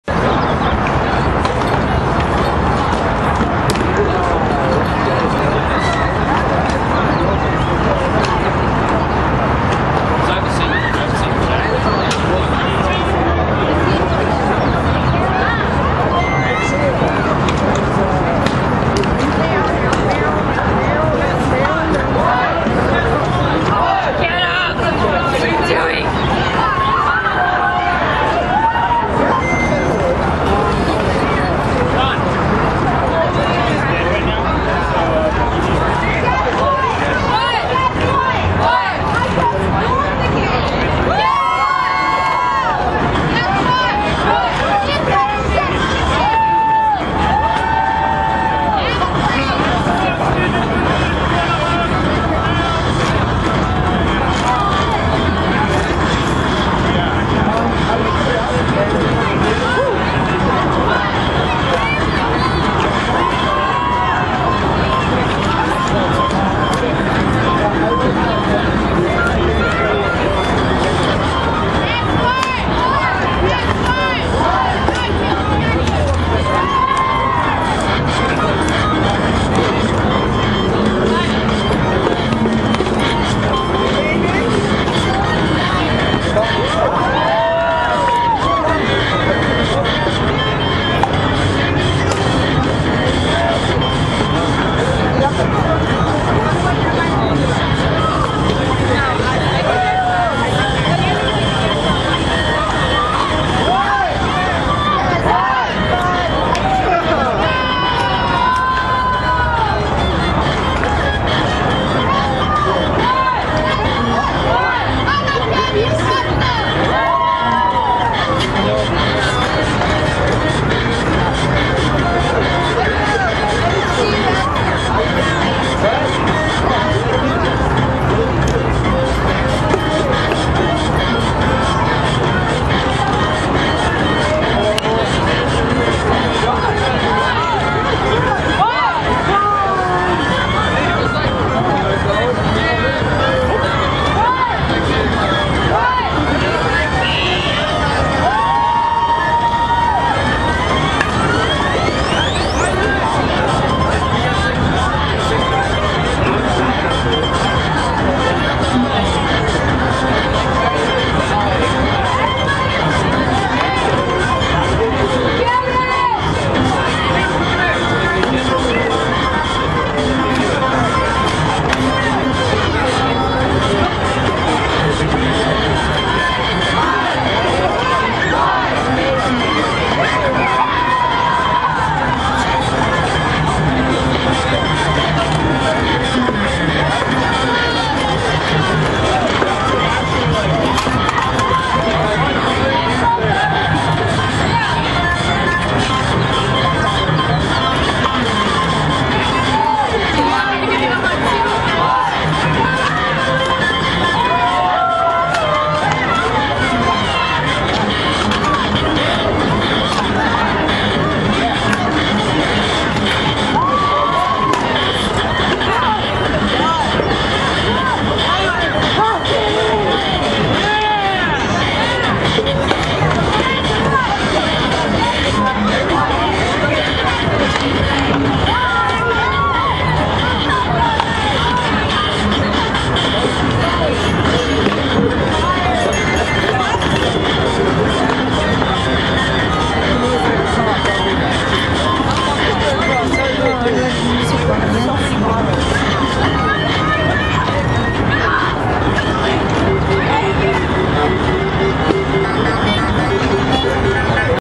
Sakura-Con 2012 The Glomping Game
Recorded on April 8th, 2012 (Sunday)

Well, this year was the first time I went to a Con. Which I have to admit, was a lot of fun. XD Even though I wasn't able to experience the entire thing. T_T My camera died on Friday, so I wasn't able to record the dating game panel. Which sucks... But I did capture this. Saturday had to be my favorite day though, which was when I became introduced to this game. I was a bit hesitant at first, but once I got over it, it turned out to be lots of fun. So many amazing cosplayers I was able to meet. ^_^